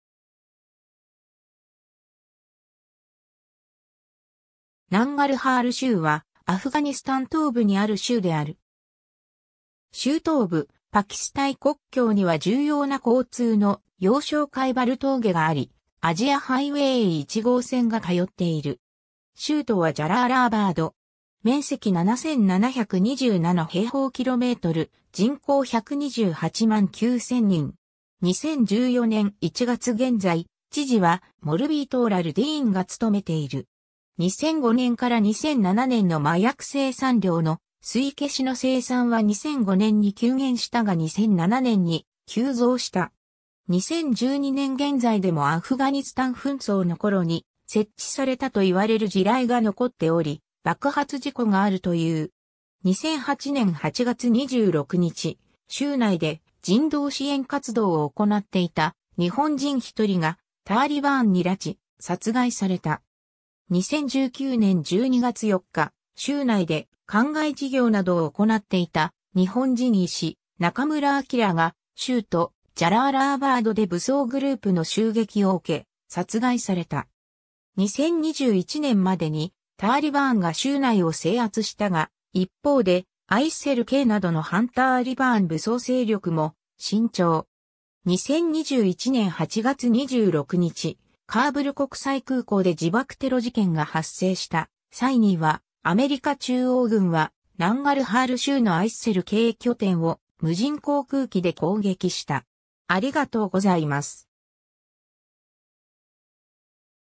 ナンガルハール州
ナンガルハール州（ナンガルハールしゅう、Nangarhār、ダリー語表記で ننگرهار）は、アフガニスタン東部にある州である。
 州東部、パキスタン国境には重要な交通の要衝カイバル峠（Khyber Pass）があり、アジアハイウェイ1号線（東京都中央区日本橋～トルコ・ブルガリア国境間）が通っている。
州都はジャラーラーバード (Jalālābād ダリー語表記でجلال‌آباد)。
面積7,727 km2、人口1,289,000人。
2014年1月現在、知事はMaulvi Attaullah Ludinがつとめている。
 2005年から2007年の麻薬生産量の推移 ケシの生産は2005年に急減したが2007年に急増した。
2012年現在でもアフガニスタン紛争 (1978年-1989年)の頃に設置されたといわれる地雷が残っており、爆発事故があると言う。
 2008年8月26日、州内で人道支援活動を行っていた日本人1人がターリバーンに拉致、殺害された。
 2019年12月4日、州内で灌漑事業などを行っていた日本人医師・中村哲が州都ジャラーラーバードで武装グループの襲撃を受け、殺害された。
 2021年までにターリバーンが州内を制圧したが、一方でISIL-Kなどの反ターリバーン武装勢力も伸長。
2021年8月26日、カーブル国際空港で自爆テロ事件が発生した際には、アメリカ中央軍はナンガルハール州のISIL-K拠点を無人航空機で攻撃した。